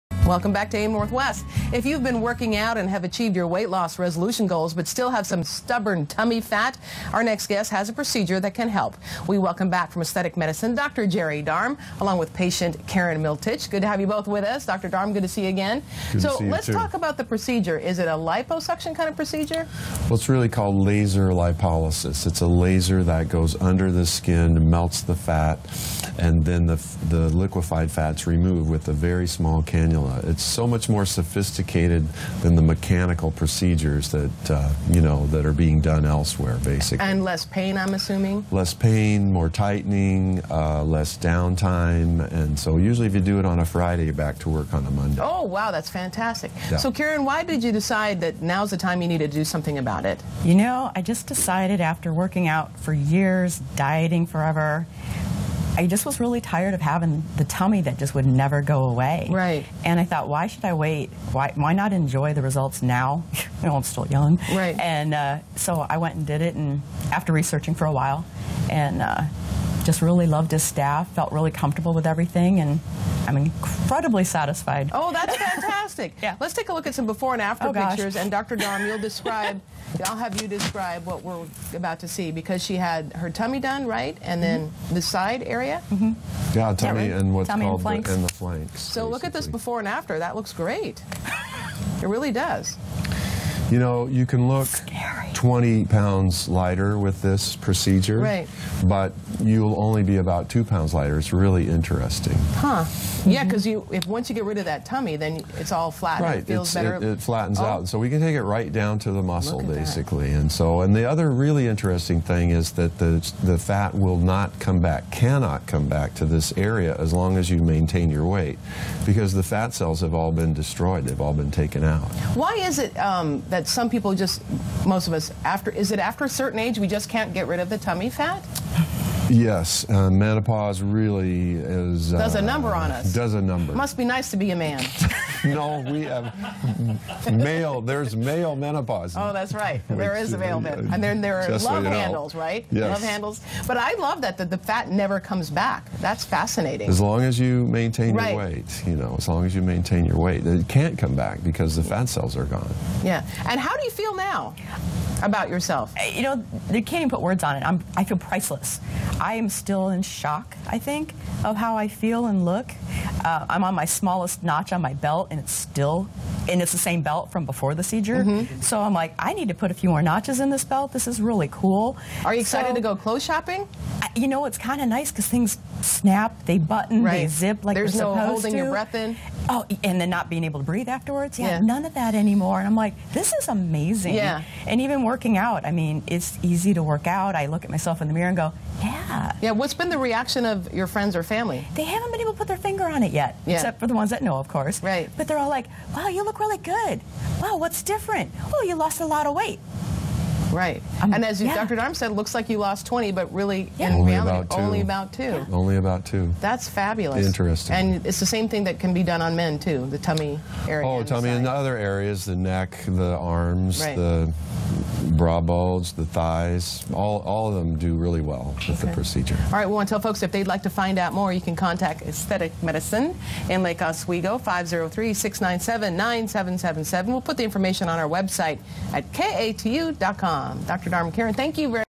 Dr. Darm LipoLift Patient Testimonials with Karyn
- Dr. Jerry Darm, of Aesthetic Medicine, joined us today, along with patient Karyn, to share how laser assisted liposuction can help remove that last bit of stubborn belly fat.